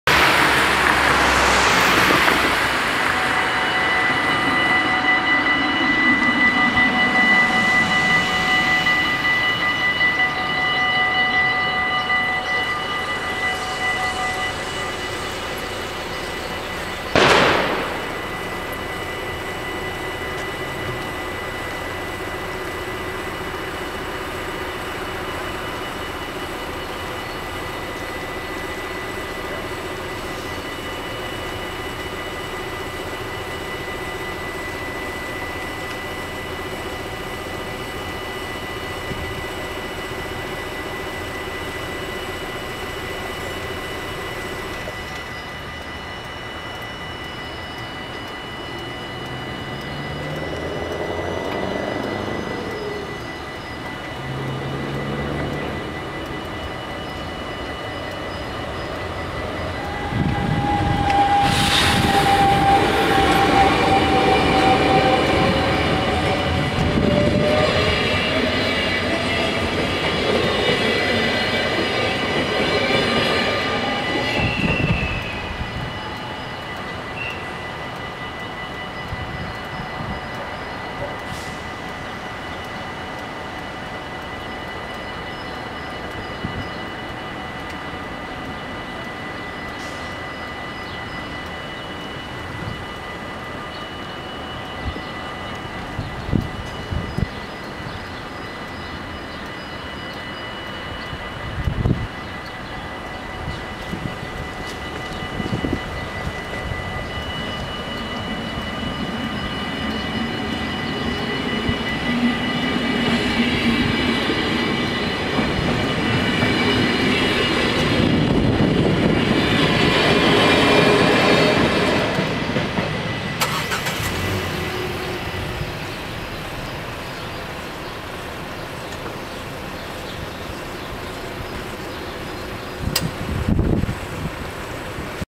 Spoorwegovergang Wezep // Dutch railroad crossing
Locatie: Stationsweg, station Wezep
Traject: Zwolle - Amersfoort
Soort: AHOB
Rode lichten: 20
Bellen: 6
Bomen: 6
Andreaskruisen: 6
Passeren:
- mDDM richting Utrecht Centraal
- mDDM richting Zwolle